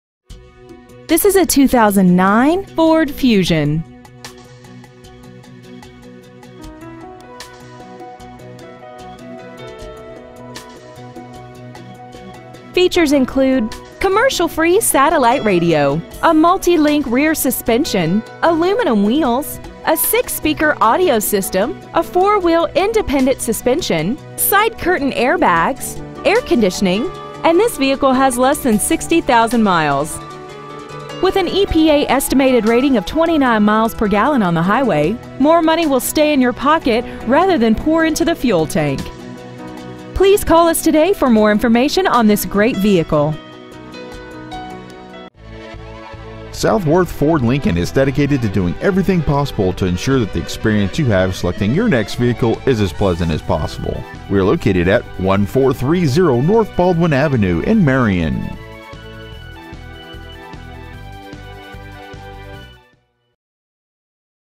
2009 Ford Fusion Marion IN
We have been honored to serve the Marion IN area , we promise that your experience at our dealership will exceed your expectations !

 Year : 2009
 Make : Ford
 Model : Fusion
 Engine : 2.3L I4 16V MPFI DOHC

 Exterior : BLUE
 Miles : 59,522

 Stock : X6431

 1430 N. Baldwin Ave
 Marion , IN 46952